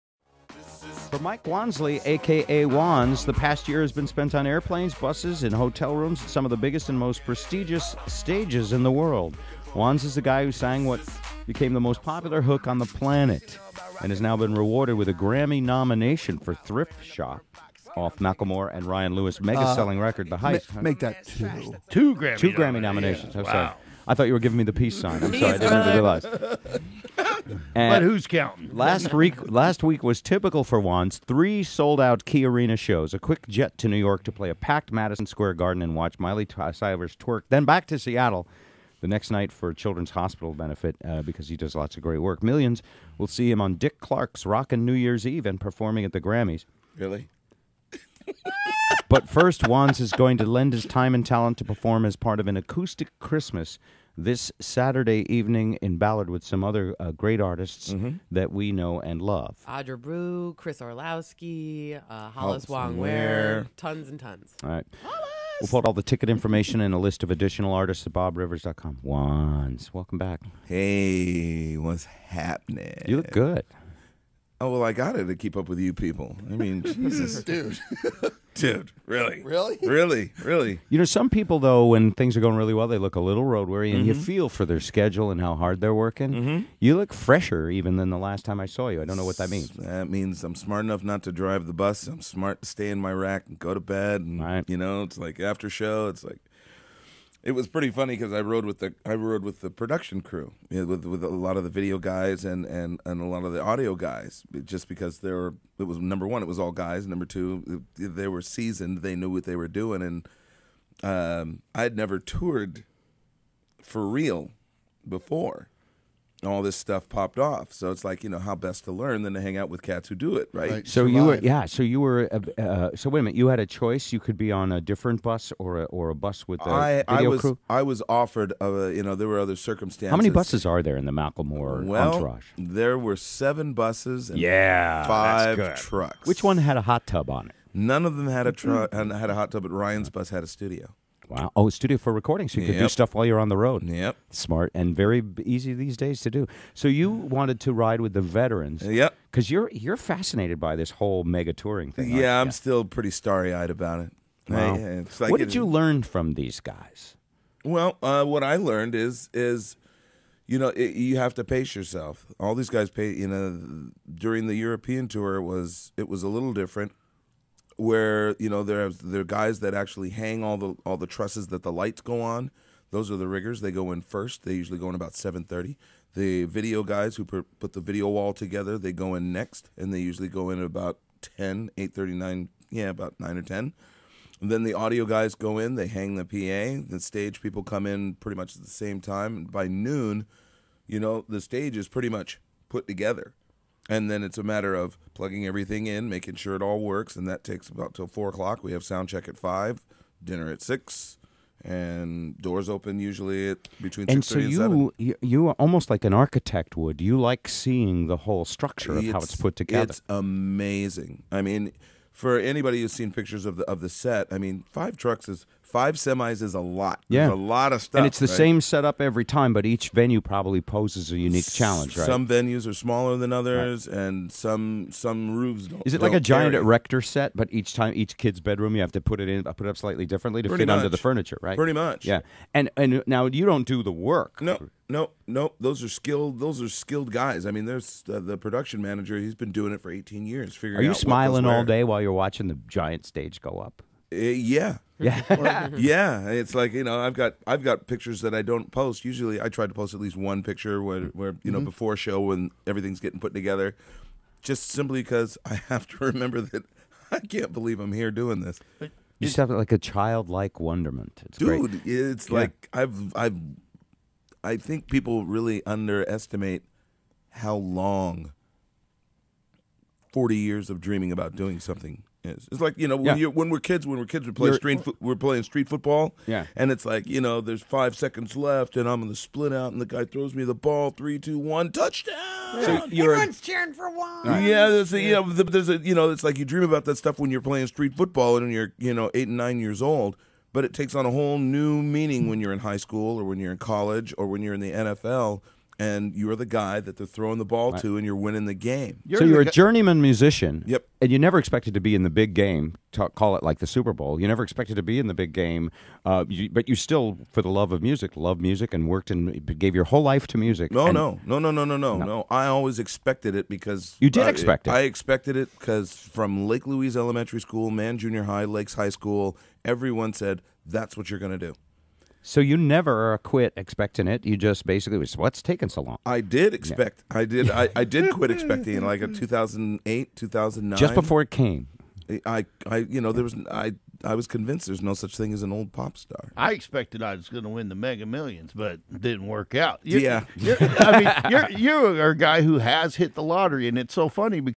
Wanz Interview (Part I) | BobRivers.com
Seattle Artist and Macklemore & Ryan Lewis cohort stopped by the studio for an amazing three part interview.